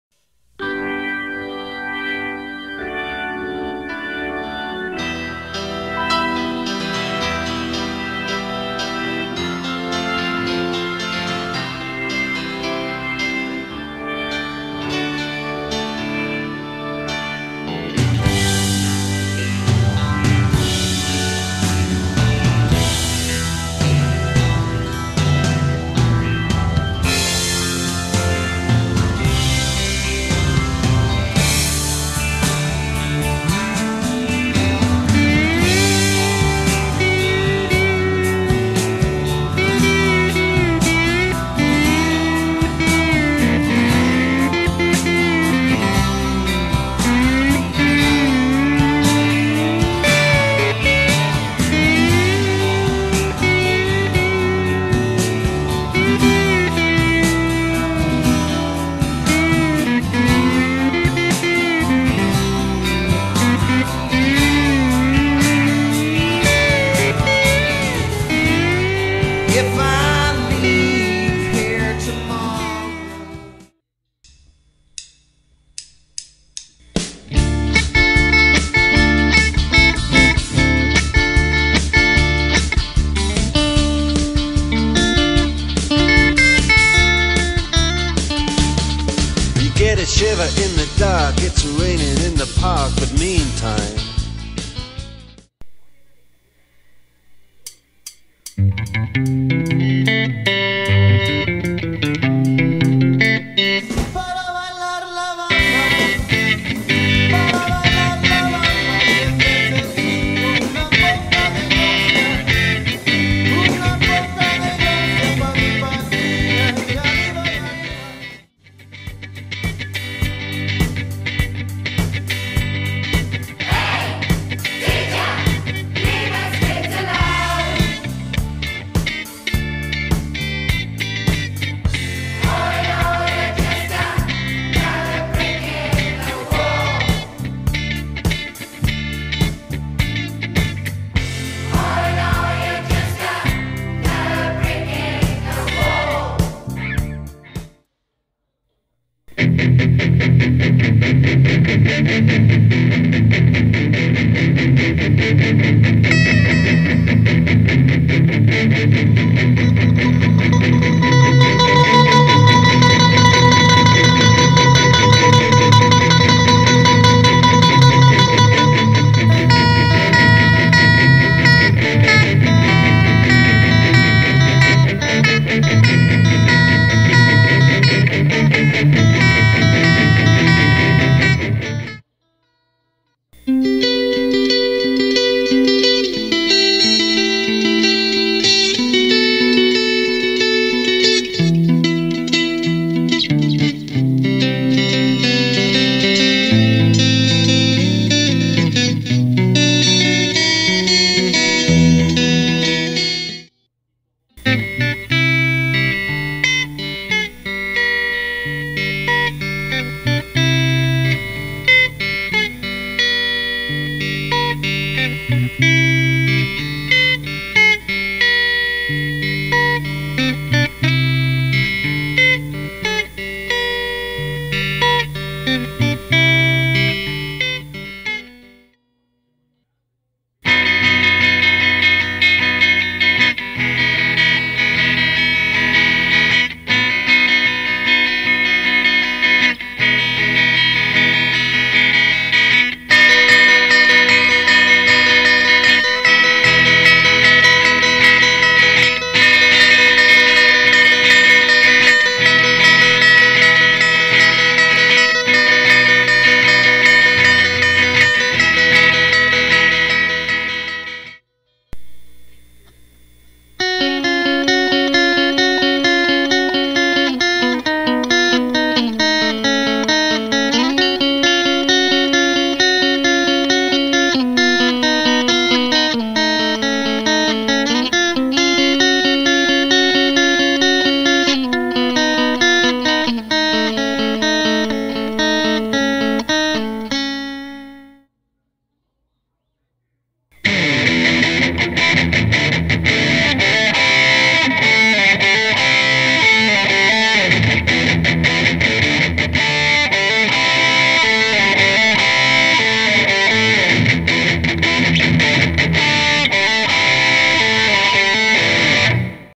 Concurso Cultural Santo Angelo Riffs Imortais - Hugo Gonçalves
Está aí a minha versão para o Concurso Cultural Santo Angelo Riffs Imortais, espero que gostem!